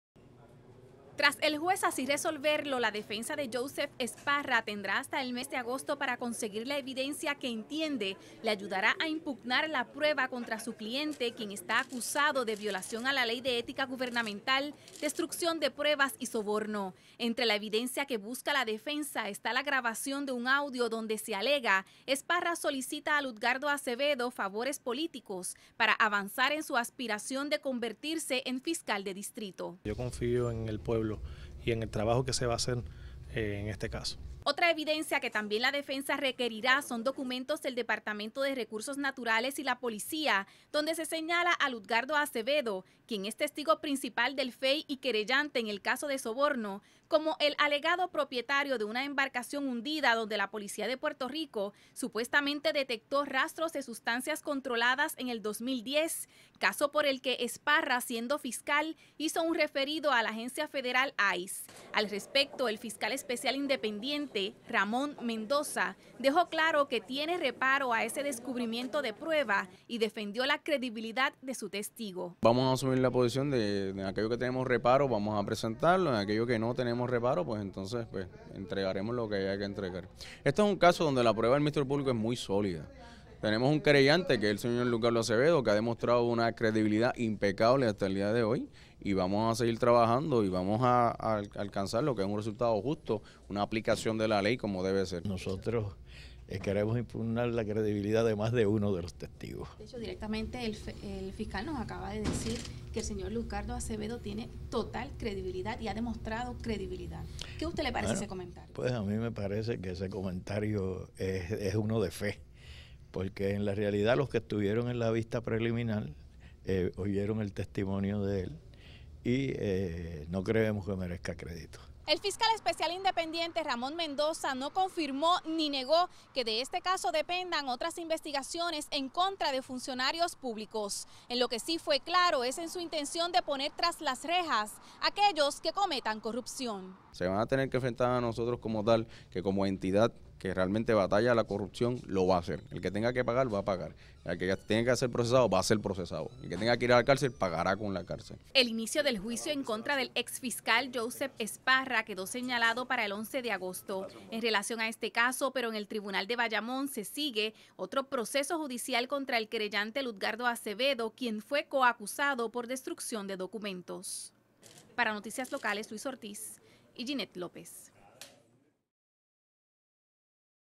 Extienden Tiempo para Lectura de Acusación de Ex Fiscal
El Juez Lynd Merle Feliciano extendió el tiempo para que se lleve a cabo el descubrimiento de prueba en el juicio que se sigue contra el Ex Fiscal Interino de Aguadilla Joseph Esparra. Esto tras la defensa presentar varias mociones solicitando al juez una órden para obtener evidencia de agencias del gobierno que permitan impugnar la prueba contra Esparra.